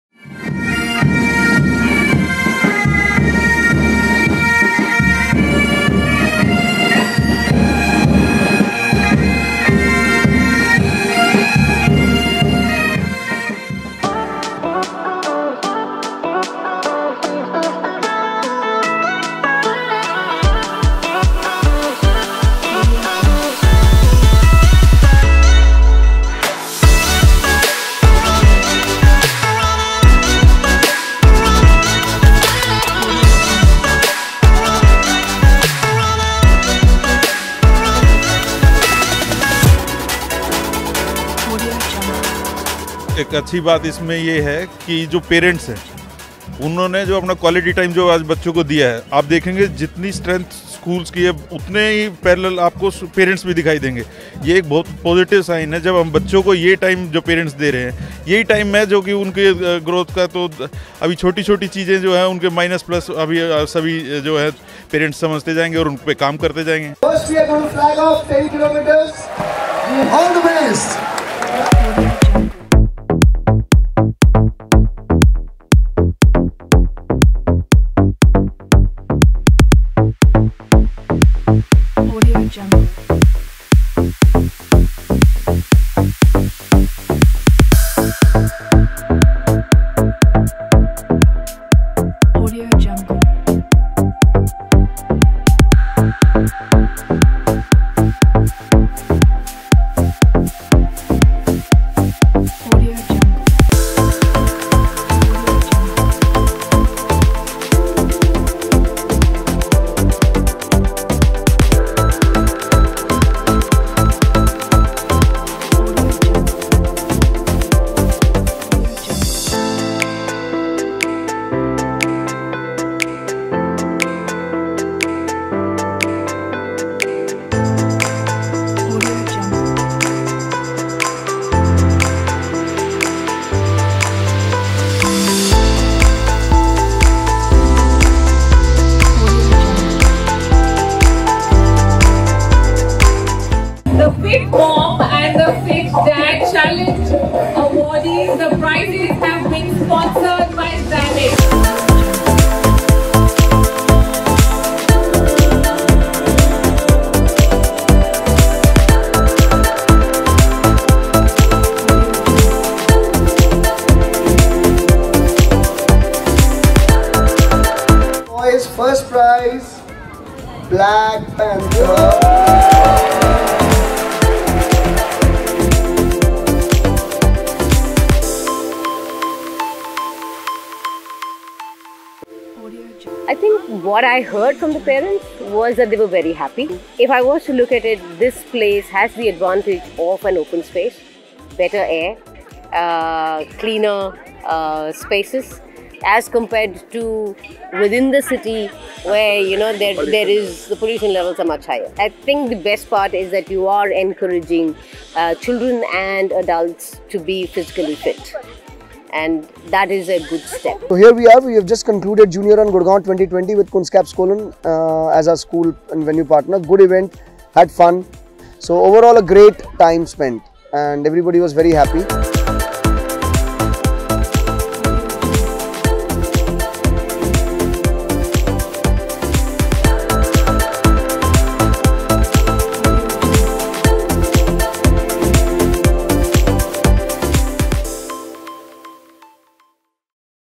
Showreel - Juniorun Gurgaon 2020
🏃‍♀Juniorun Gurgaon 2020 unfolded new areas of running for kids. Kids had a great time running in open areas of 🏫Kunskapsskolan International School, Gurgaon (School Partner) breathing fresh air and having a great time with their parents as well.

📺Catch the glimpses of Juniorun Gurgaon 2020.

📲 Keep sharing.
👍 Keep Supporting.